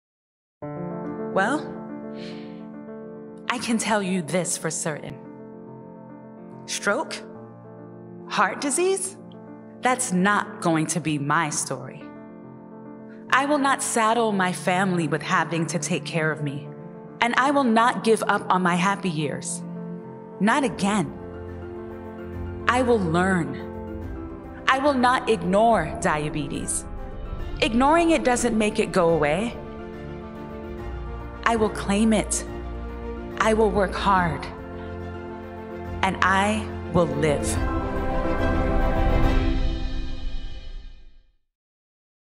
Claiming a Type 2 Diabetes Diagnosis
Learn more about type 2 diabetes and its connection with heart disease, including balanced recipes

 The American Heart Association and the American Diabetes Association, along with industry leaders, have proudly launched the groundbreaking collaborative initiative Know Diabetes by Heart™ to reduce cardiovascular deaths, heart attacks and strokes in people living with type 2 diabetes.